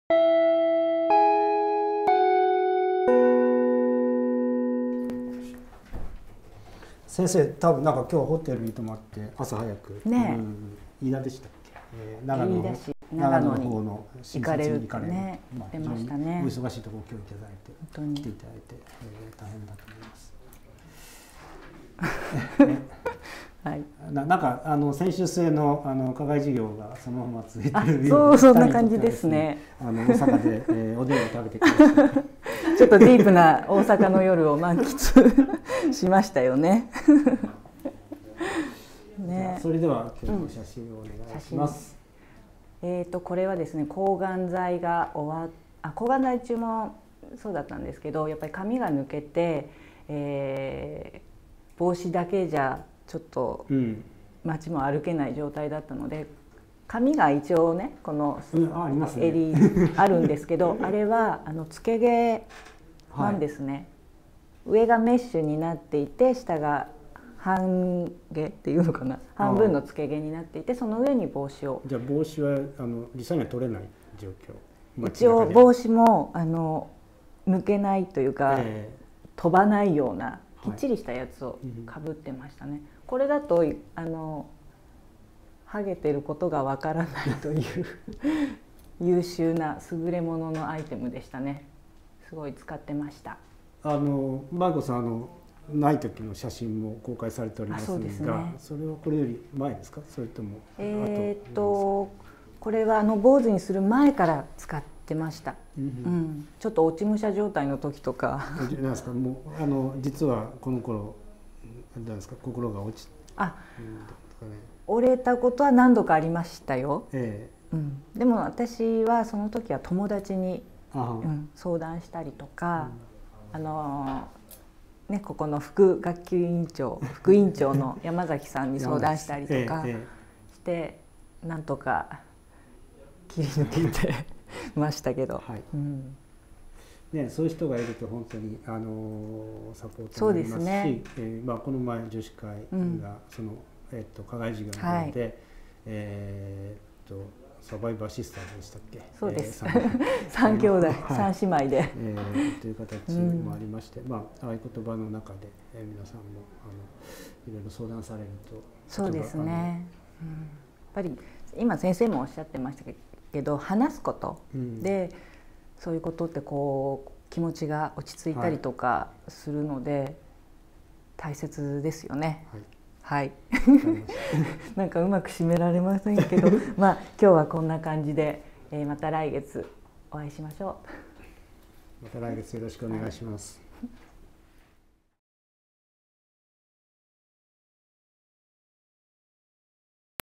アピタル乳がん夜間学校 第8回目-放課後「抗がん剤中のMAIKOさん」
2011年11月16日（水）に生放送したアピタル乳がん夜間学校 第8回「乳がんの薬物療法（ホルモン剤）について」放課後「 抗がん剤中のMAIKOさん」。
先生は、埼玉医科大学国際医療センター　腫瘍内科 教授　 佐々木康綱先生。 日直（司会）はおなじみ、モデルのMAIKOさん サポート役は学級委員長（アピタル編集長）の平子義紀さんです。

これまでのアーカイブは「アピタル乳がん夜間学校　インデックス」へ

もっとがん医療の映像を見たい方はキャンサーチャンネルへ